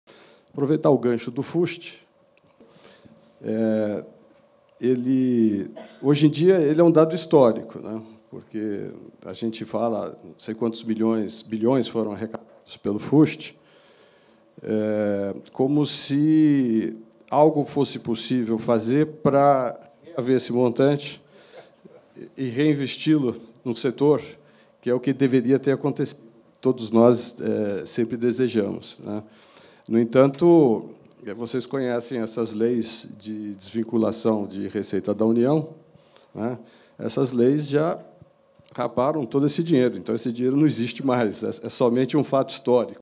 Impossível pensar em recuperar o dinheiro do FUST para investir na banda larga
"Esqueçam o FUST. Não há menor chance desse dinheiro voltar a ser reinvestido no setor", dessa forma, o secretário de Telecomunicações do Ministério da Ciência, Tecnologia, Inovação e Comunicação, André Borges, falou dos planos setoriais para a expansão da oferta de banda larga no Brasil.

Borges, que participou do 30º Seminário Internacional da ABDTIC, em São Paulo, foi taxativo: não adianta cobrar o dinheiro recolhido desde a privatização do setor e não investido no próprio setor. "Não há como recuperar esse dinheiro. O que vier do FUST daqui pra frente é uma negociação de médio e longo prazo e envolve a área econômica".

Até então, o MCTIC, e o próprio André Borges, acreditava que era possível incluir uma 'emenda Fust' no PL 3453/15, que avançou no Congresso Nacional e que permite a troca de concessões por autorizações. Mas, agora, o executivo se mostra menos otimista com o uso do FUST no novo PNBL. Mas Borges acredita ser possível usar o TAC - Termo de Acordo de Compromisso, firmado pela Anatel com as operadoras em troca das multas por não atendimento na expansão da banda larga.

"Esses recursos vão ser usados para garantir que a rede - e não necessáriamente a fibra óptica- chegue nos municípios onde as teles não querem investir. Essas são áreas que queremos atendidas e serão razão do novo PNBL", atestou André Borges. A transformação do PL 3453/15 em Projeto de Lei ainda em 2016 é, admite o secretário do MCTIC, é 'otimismo', mas a iniciativa é que está endossada pelo governo para acelerar a revisão do setor de Telecomunicações.